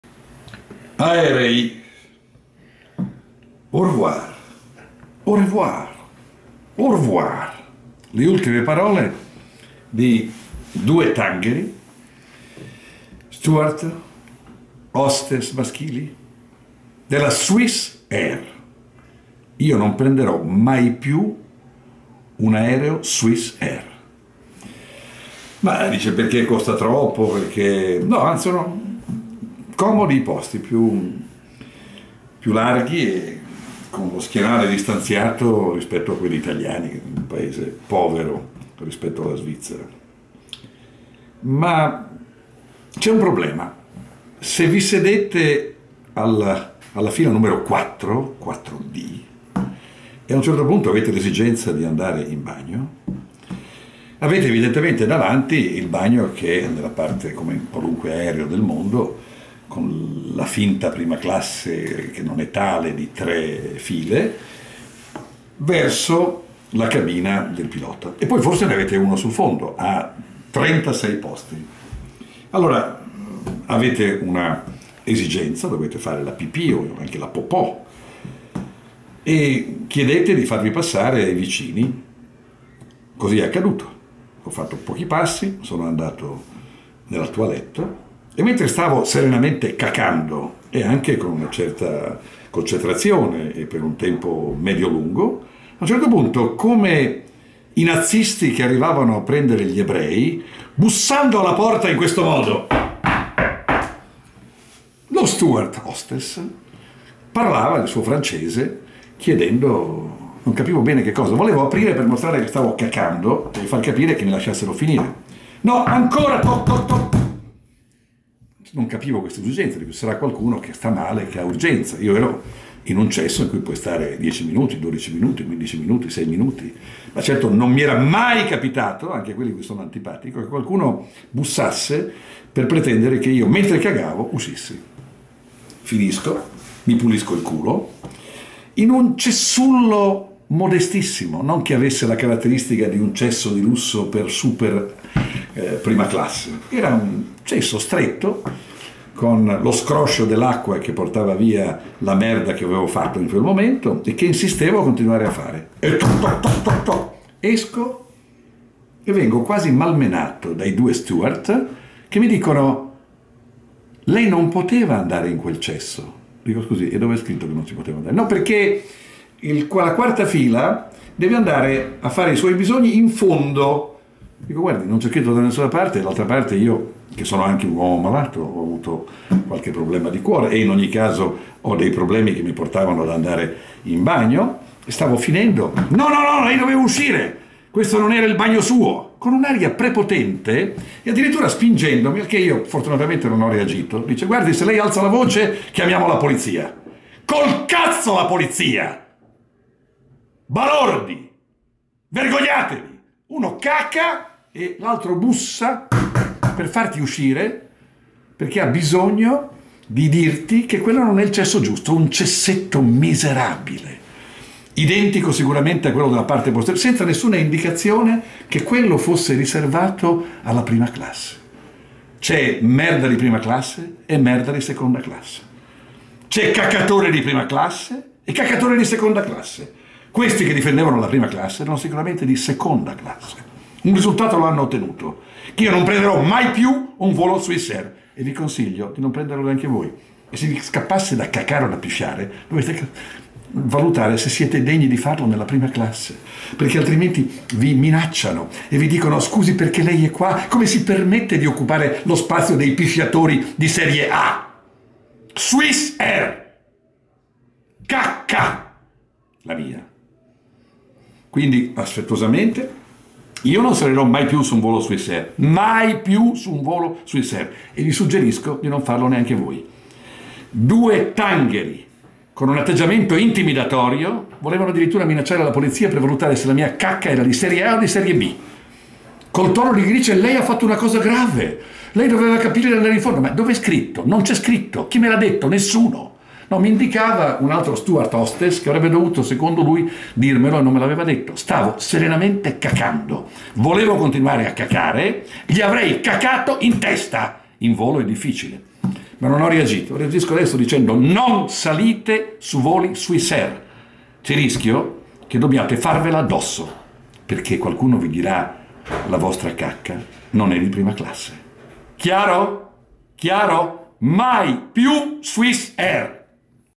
Sgarbi: "Mai più su un volo della Swiss, dove ci sono "cacatori" di prima classe e seconda classe"
La disavventura del critico d'arte sul volo Roma-Ginevra del 18 settembre 2016.